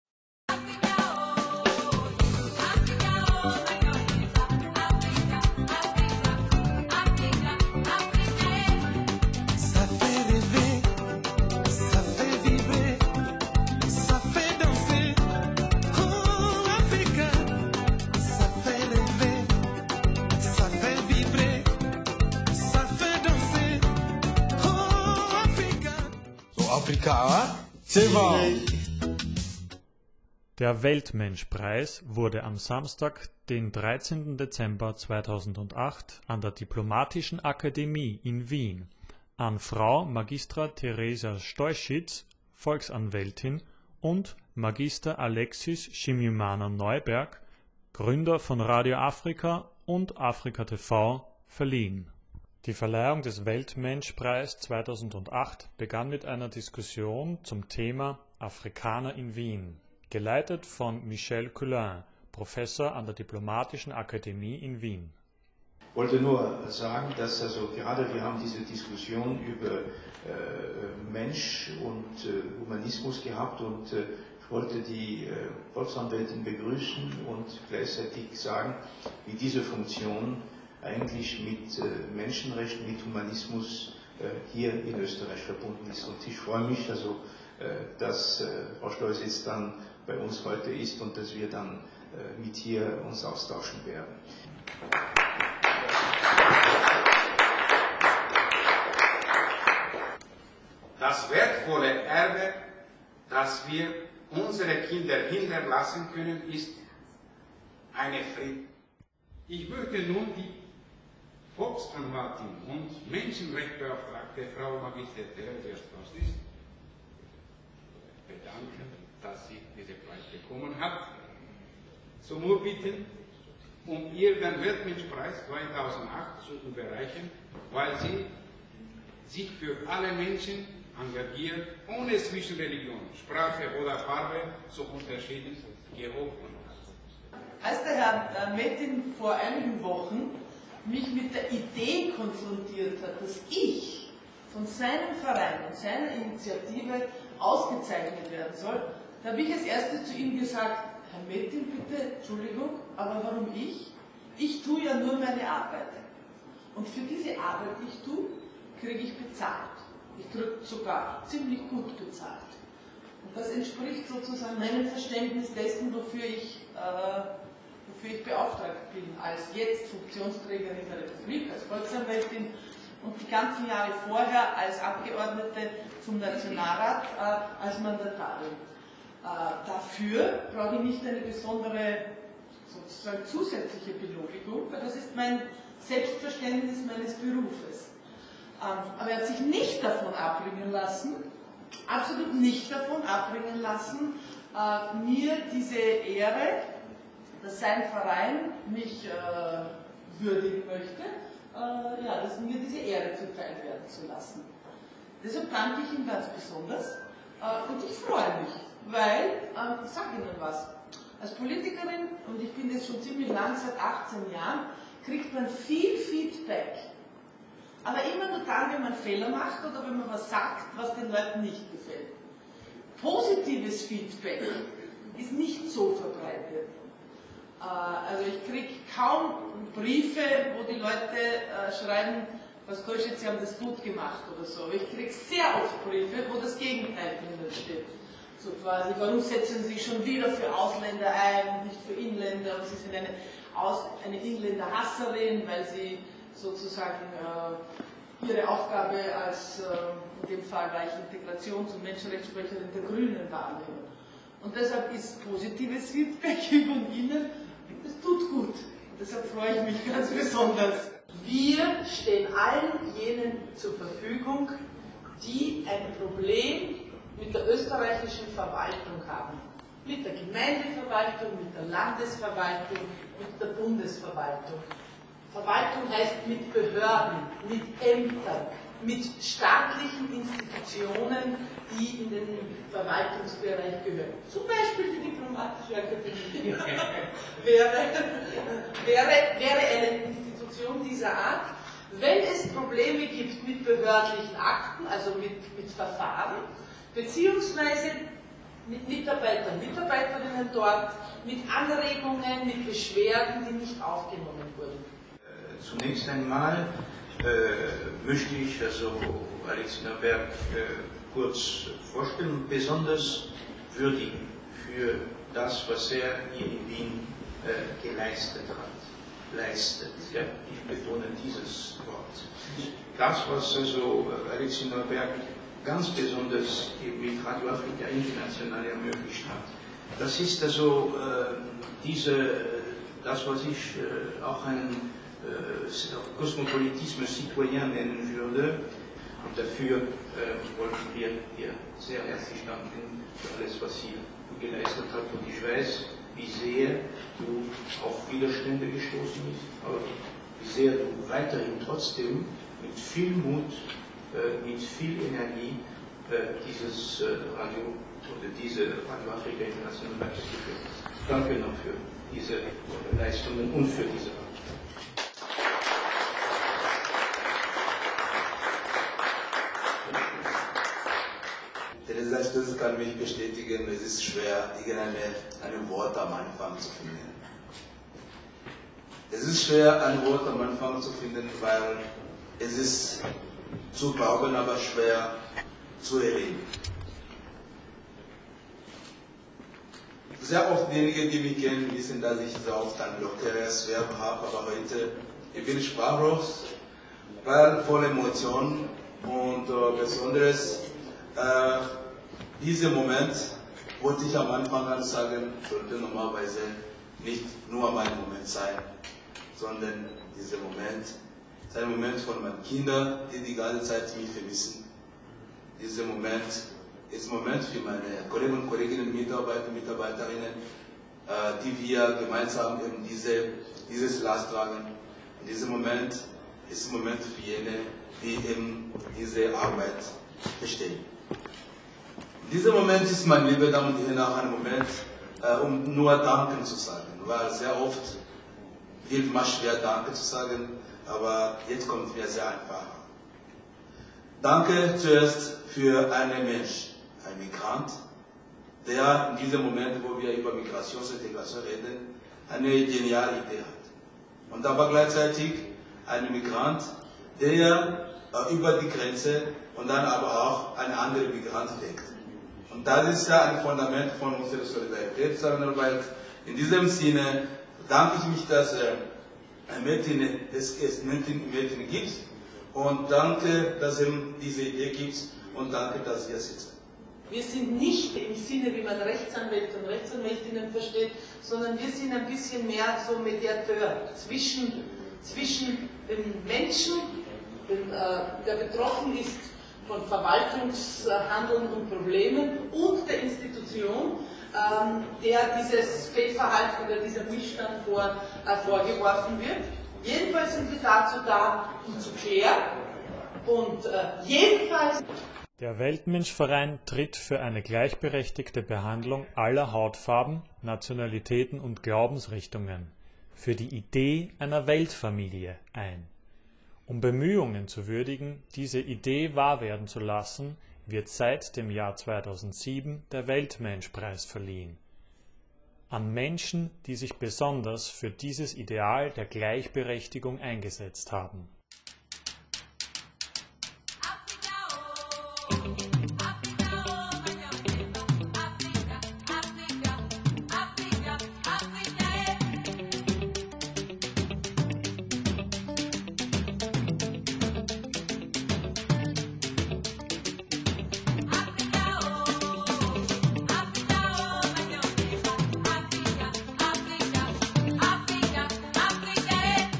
Weltmenschpreis 2008 final
Eines der Ziele des WMV ist der Weltmensch-Preis, der seit dem Jahr 2007 an herausragende und verdienstvolle Leistungen für die Menschheit verliehen wird. Am 18. Dezember 2008 wurde dieser Preis zum zweiten Mal verliehen, diesmal an Alexis Neuberg und Terezija Stoisits.